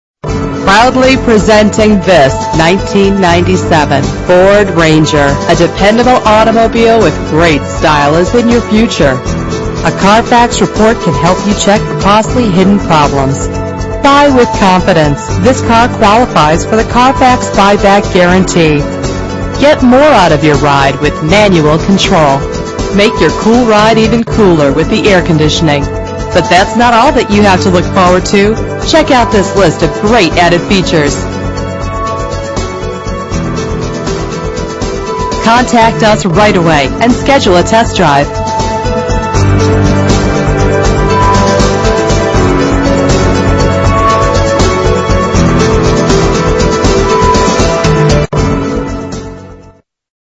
1997 Ford Ranger in Atlanta Lawrenceville, GA
719 W. Pike St
Atlanta Lawrenceville, GA 30046

1997 Ford Ranger
5-Speed Manual
Mileage:178076